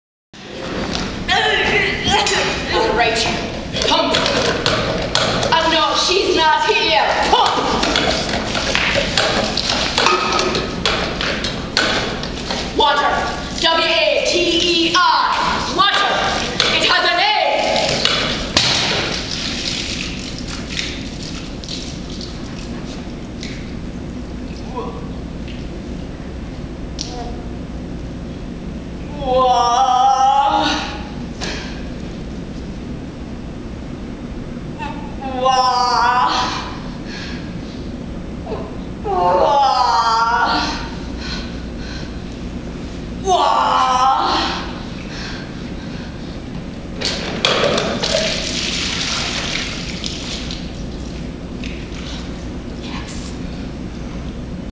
Miracle Worker - with Mary McGahey
Miracle Worker - with Mary McGahey and Patricia Bradford, April 5, 2007.